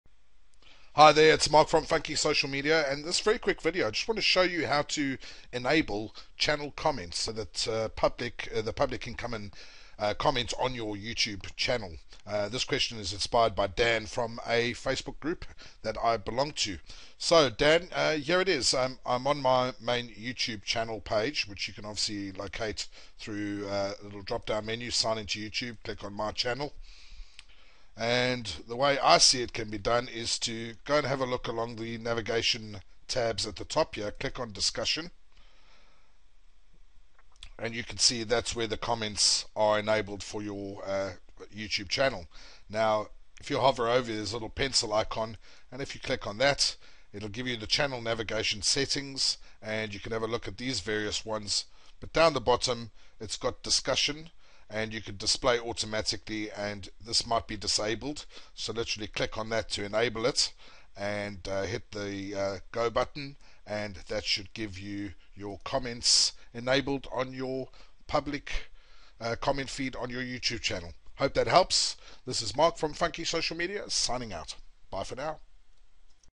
How To Enable Your Youtube Channel Comment Feature
Had a question asked in a Facebook group I belong to on how to enable your YouTube channel comment feature so that members of the public can post comments. I've explained how to do that in this video.